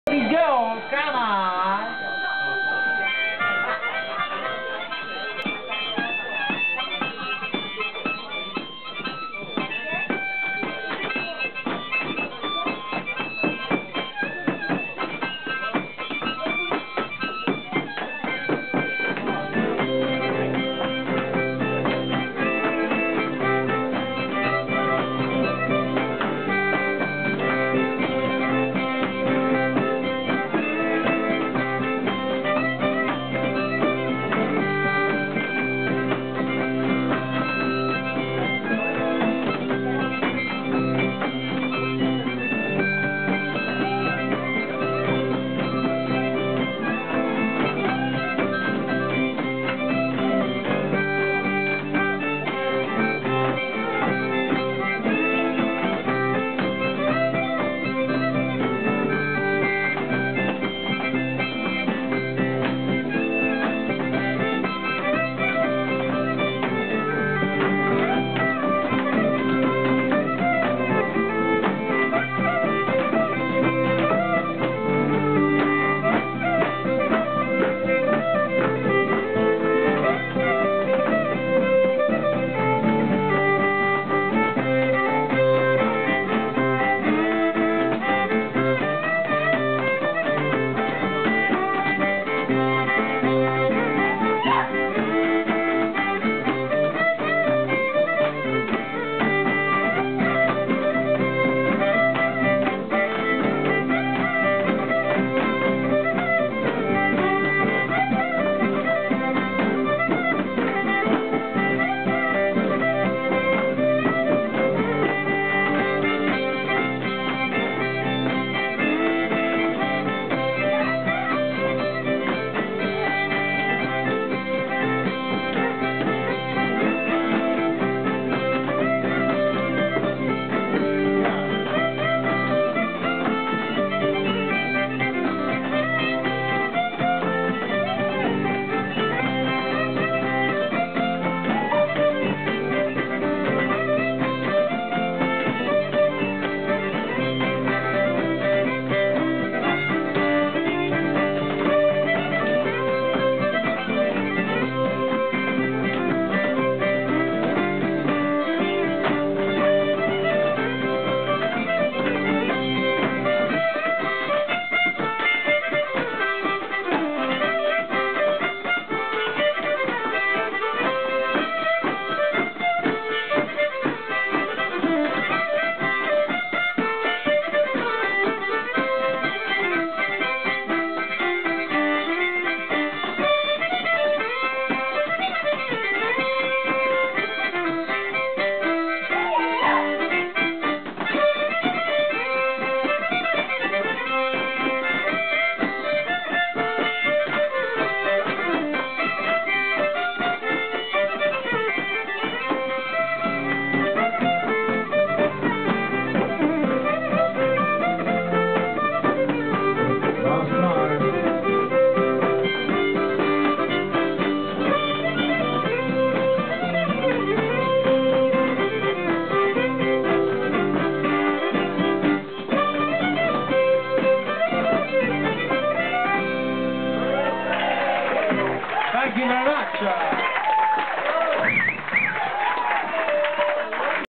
2010-05-31 - Dindon Sauvage - Hook+Hatchet at Hucking - P1080887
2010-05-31 Dindon Sauvage, with Yves Langlois on fiddle and multi-instrumentalist Chris Taylor  at the Hook+Hatchet at Hucking, with the Boardwalk Hoofers doing an Appalachian dance